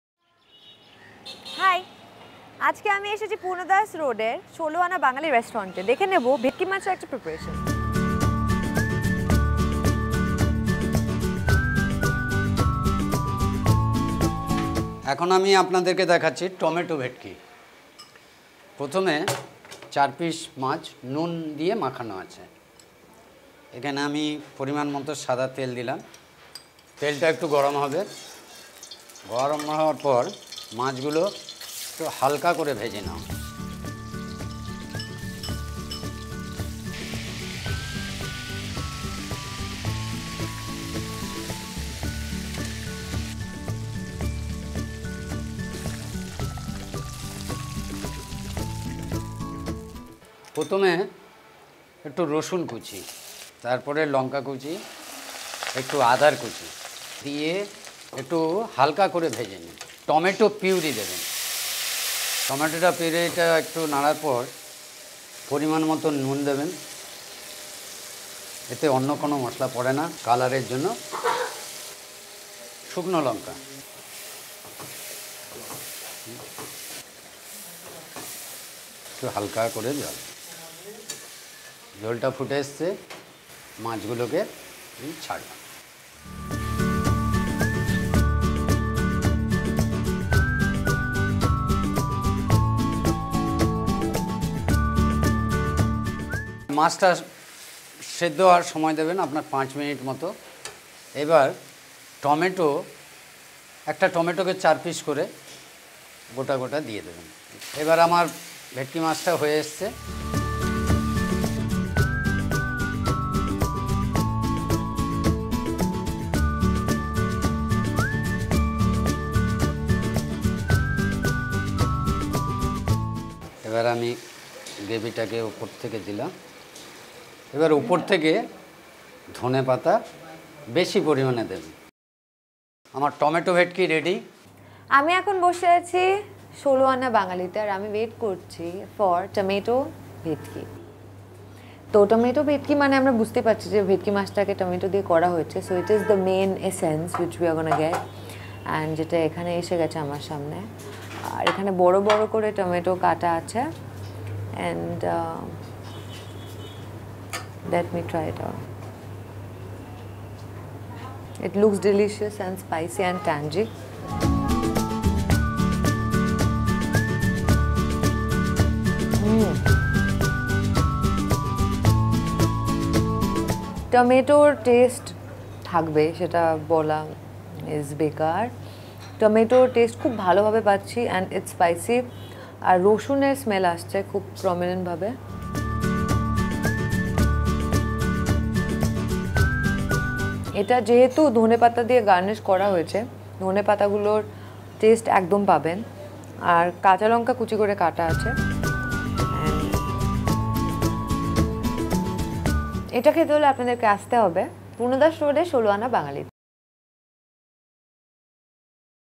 Tomato Bhetki Maach (Fish) | Sholo Ana Bangali | Kolkata | Bengali Cuisine
"Sholo Ana Bangali"in Purna Das Road, Kolkata.

Ingredients of "Tomato Bhetki":
Bhetki  fish (4 pcs), White oil, Chopped garlic, Chopped green chilli, Chopped ginger, Tomato puree, Salt, Chilli powder,  whole tomato (1) and Coriander leaves.